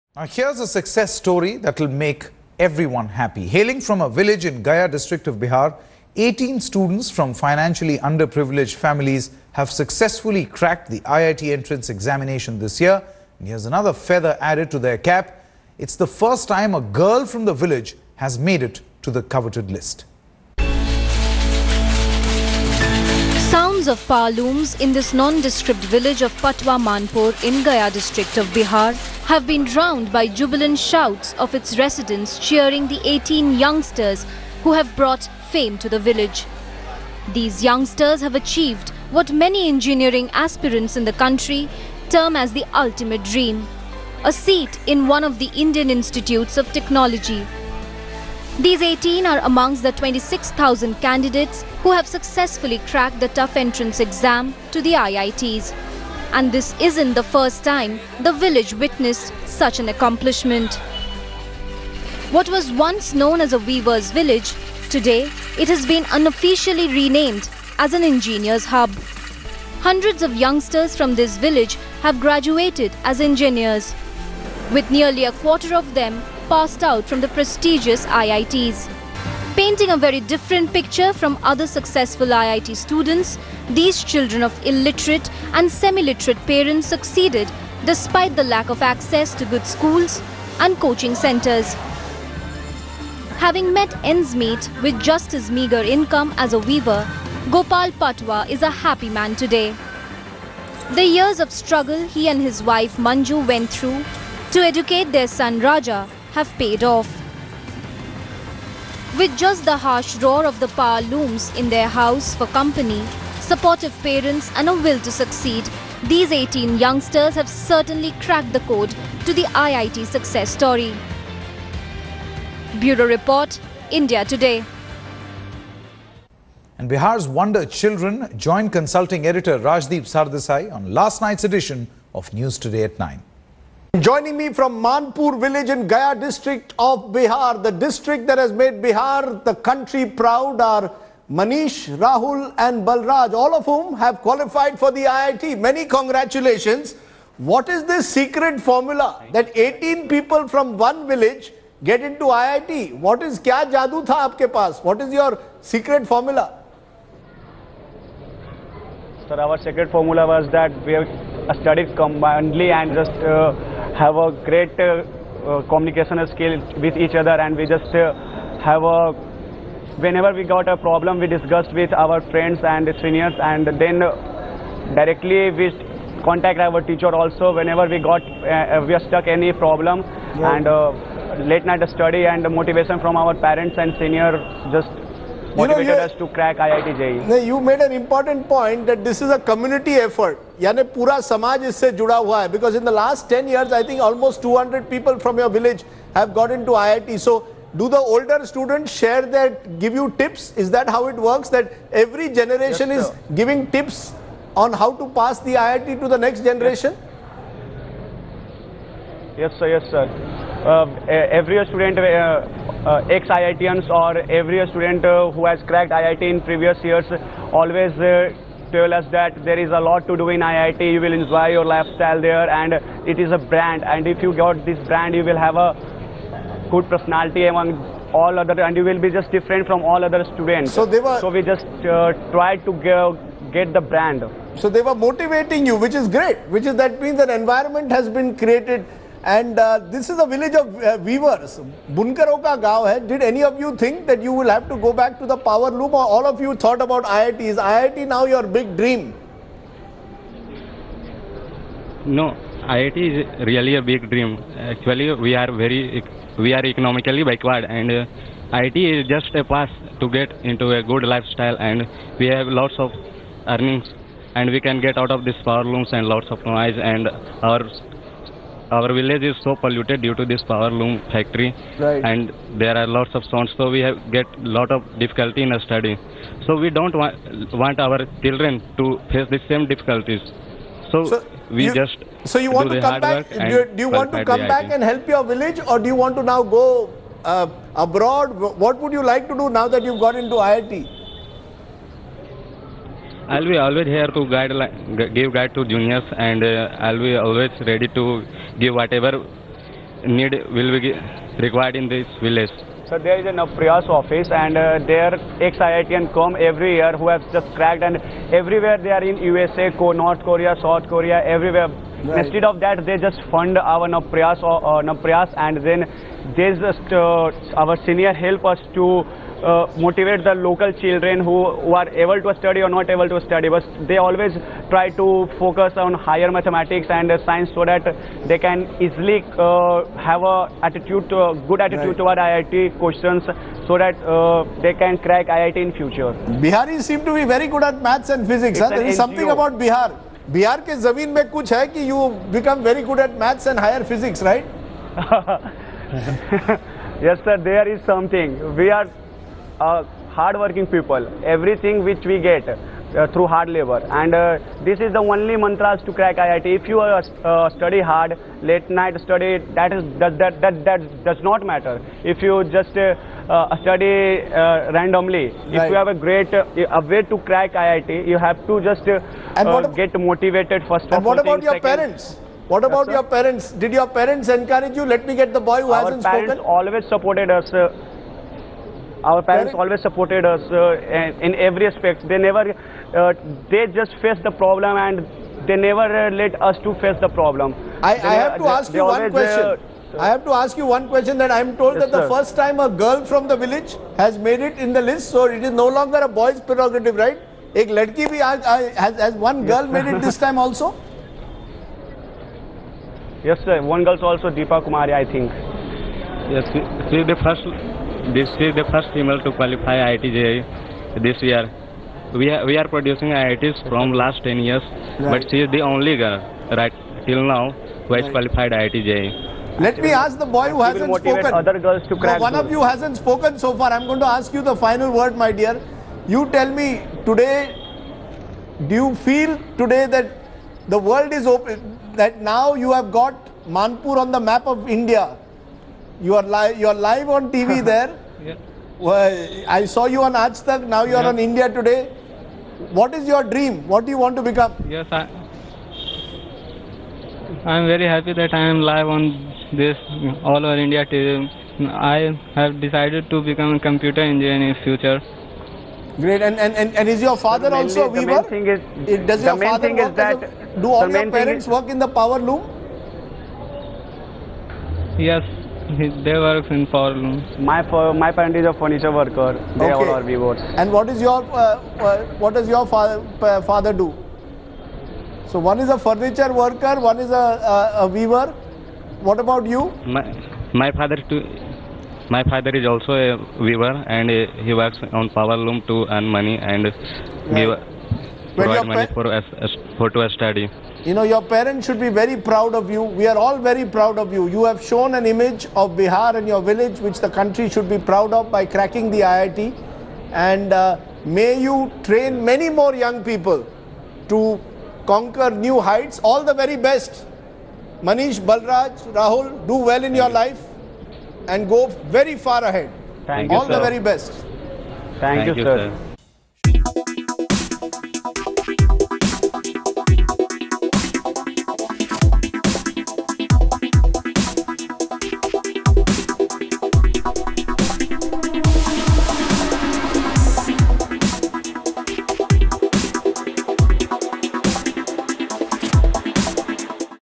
18 Bihar Students Crack IIT Entrance Examination
Hailing from a village in Bihar, 18 students from financially under privileged families have successfully cracked the IIT entrance examination this year.